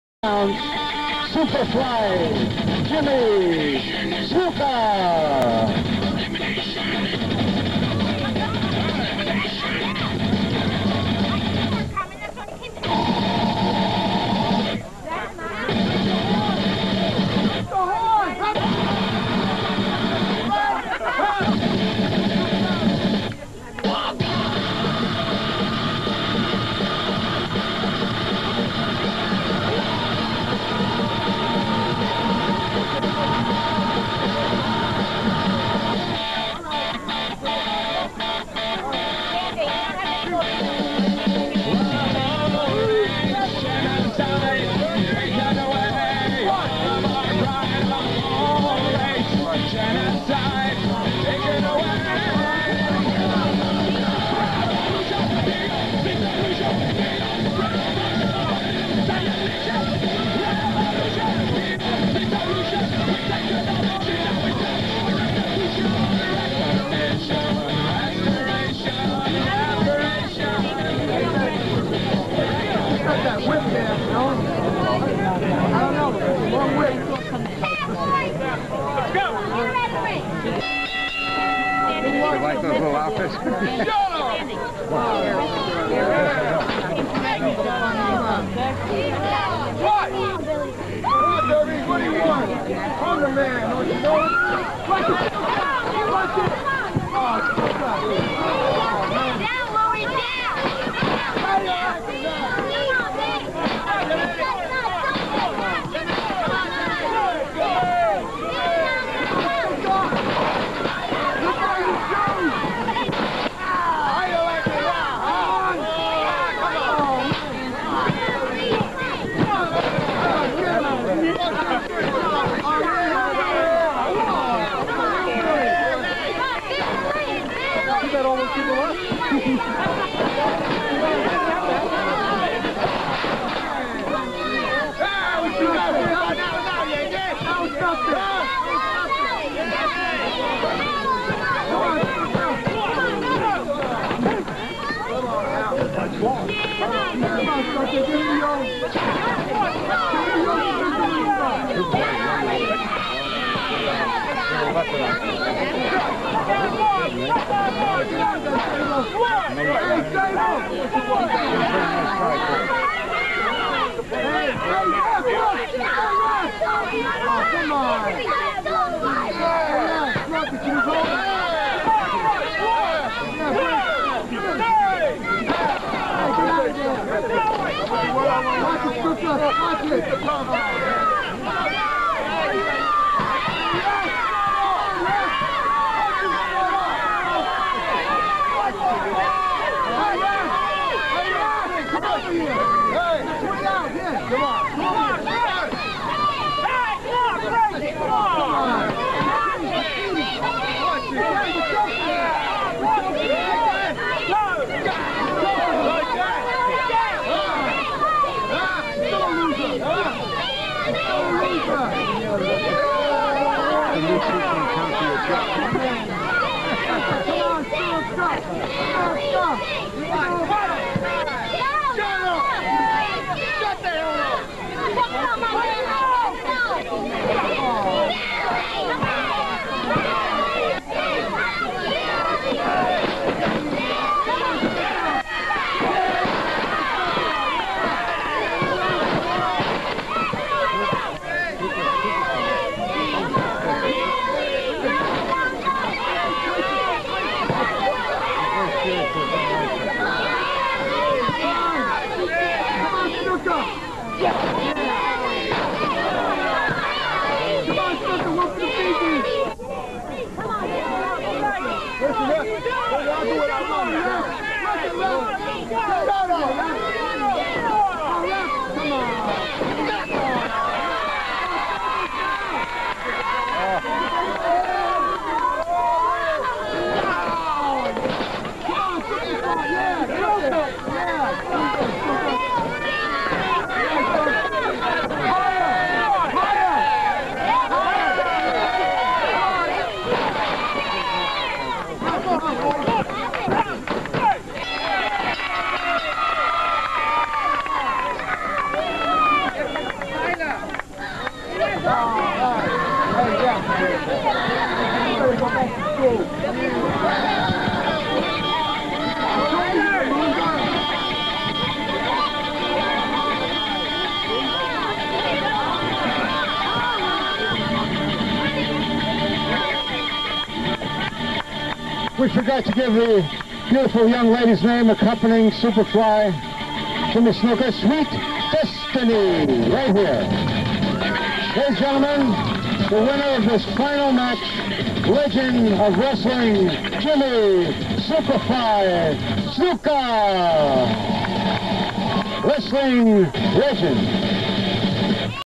Bad Billy Walker vs Jimmy SuperFly Snuka, WUW 2000
From Little Ferry, NJ
World Of Unpredictable Wrestling own's Bad Billy Walker w/Texas Rose faces off with WWE Hall of Famer Jimmy "Superfly" Snuka (with valet Lil Jeanie/Sweet Destiny)